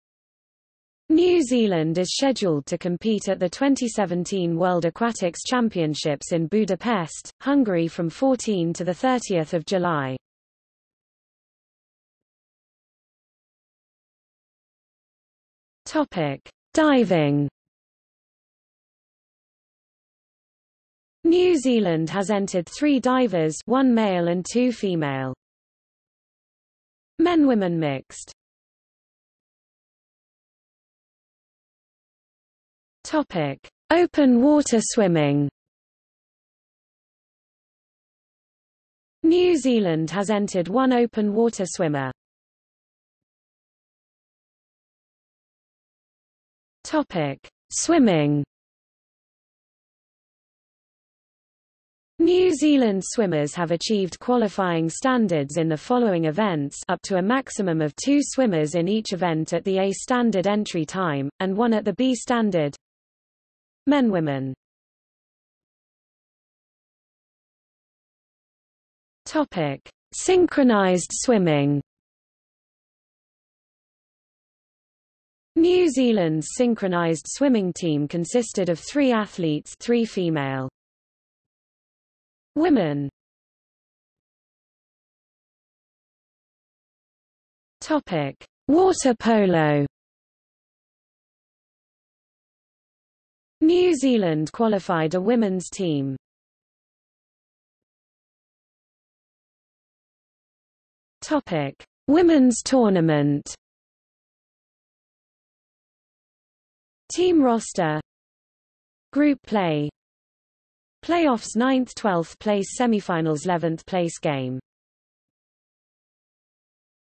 New Zealand at the 2017 World Aquatics Championships | Wikipedia audio article
00:00:12 1 Diving
00:00:29 2 Open water swimming
00:00:43 3 Swimming
00:01:05 4 Synchronized swimming
00:01:24 5 Water polo
00:01:37 5.1 Women's tournament

- Socrates

SUMMARY
New Zealand is scheduled to compete at the 2017 World Aquatics Championships in Budapest, Hungary from 14 to 30 July.